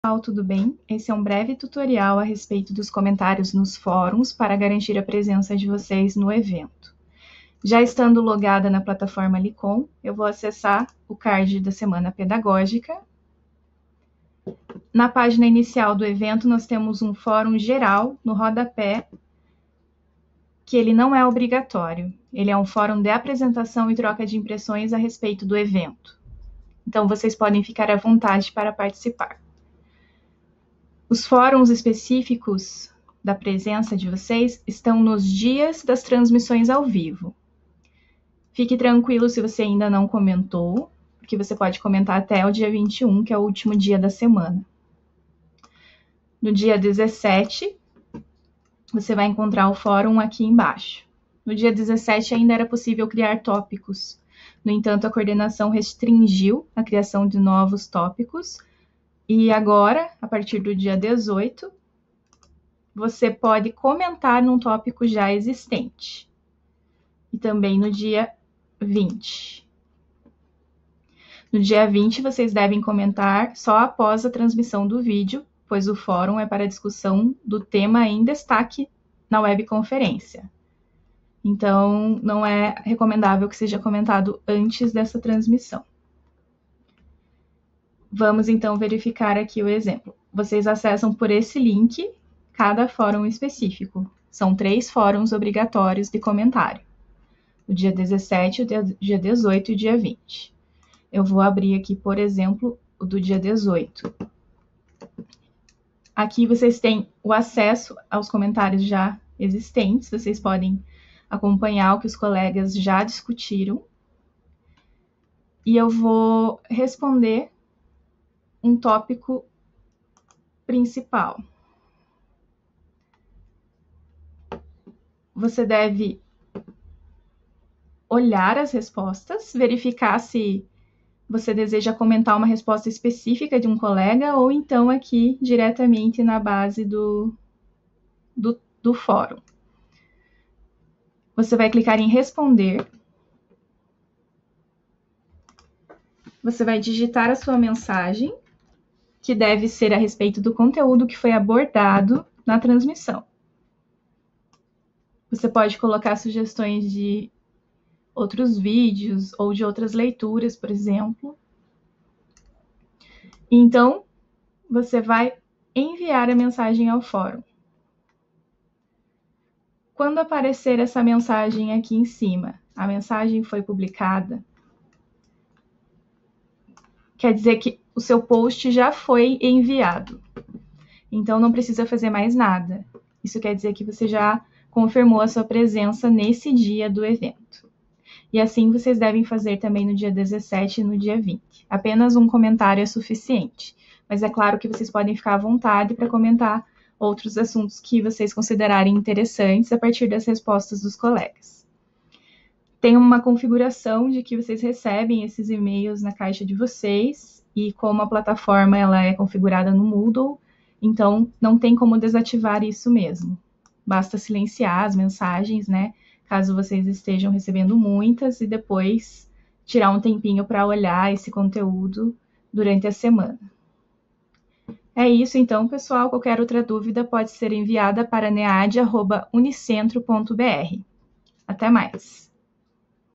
[TUTORIAL] Fórum - Semana Pedagógica Interfaces Digitais e Educacionais
Passo a passo sobre como comentar nos fóruns da Semana Pedagógica Interfaces Digitais e Educacionais e marcar a presença nas atividades do evento.

Tutorial por Scheyla J. Horst, comunicadora social do Nead/UAB Unicentro.